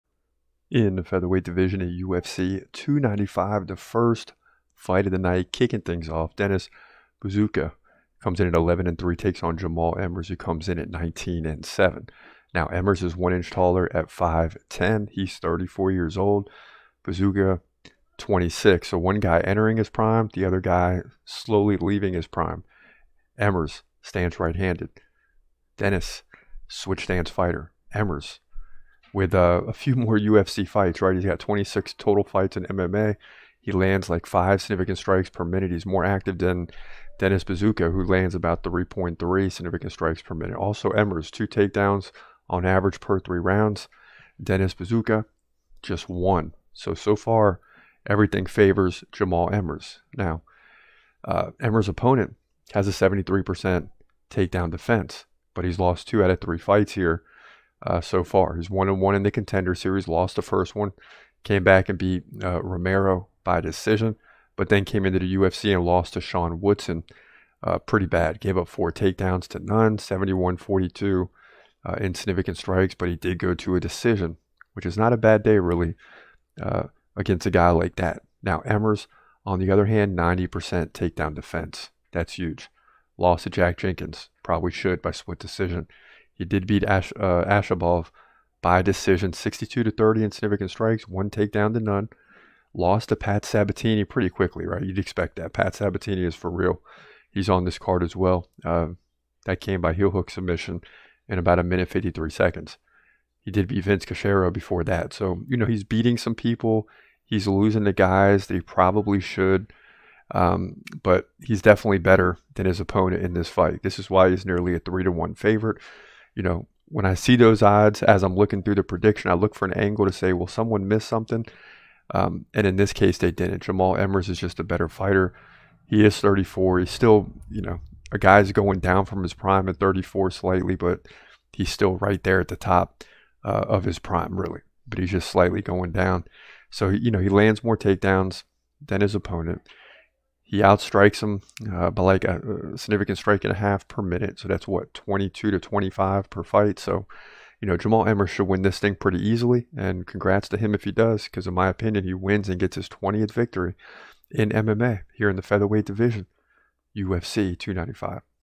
UFC 295: Dennis Buzukja vs Jamall Emmers PREDICTION
@ChrisCrossMMA predicts the winner of Dennis Buzukja vs Jamall Emmers at UFC 295...